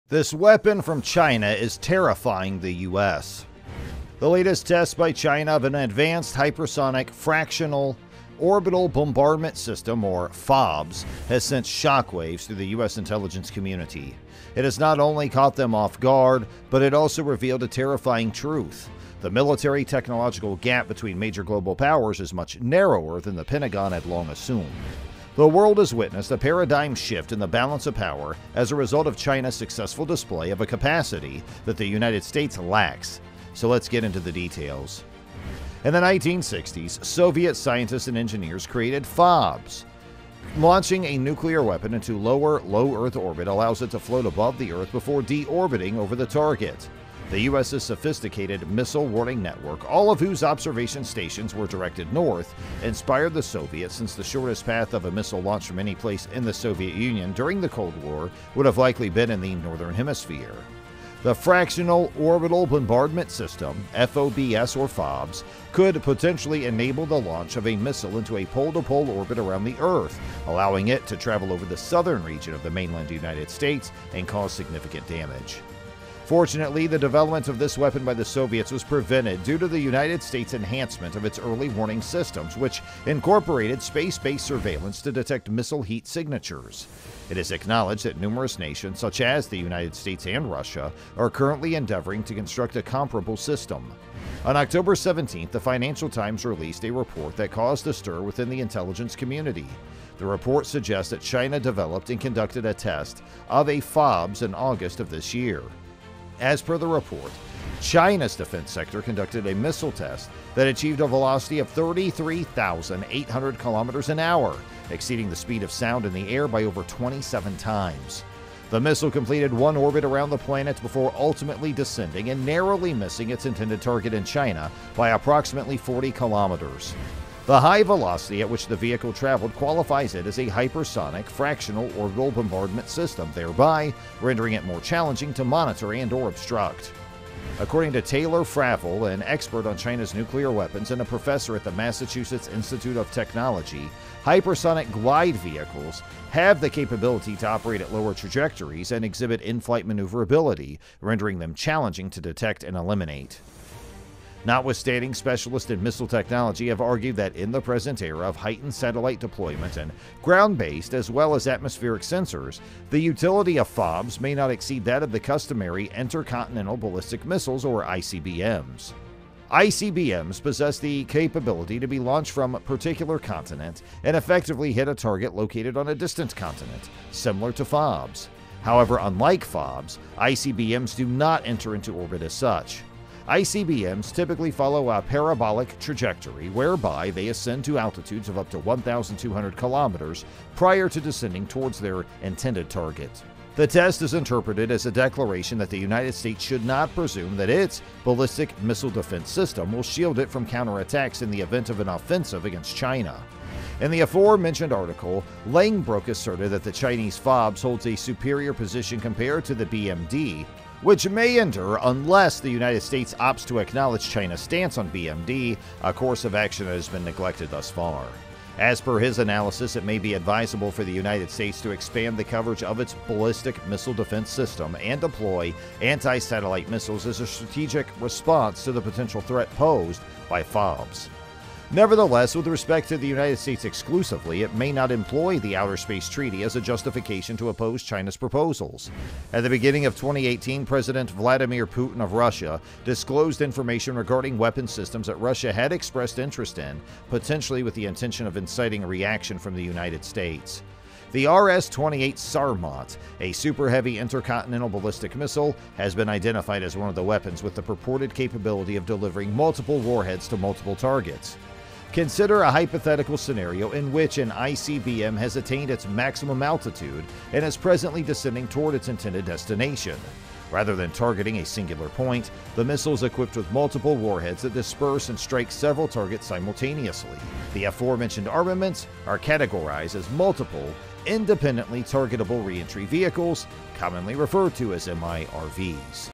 This Weapon from China is Terrifying the US!
The latest test by China of an advanced hypersonic fractional orbital bombardment system (FOBS) has sent shockwaves through the US intelligence community. It not only caught them off guard, but it also revealed a terrifying truth: the military-technological gap between major global powers is much narrower than the Pentagon had long assumed. The world has witnessed a paradigm shift in the balance of power as a result of China's successful display of a capacity that the United States lacks. Let's get into the details.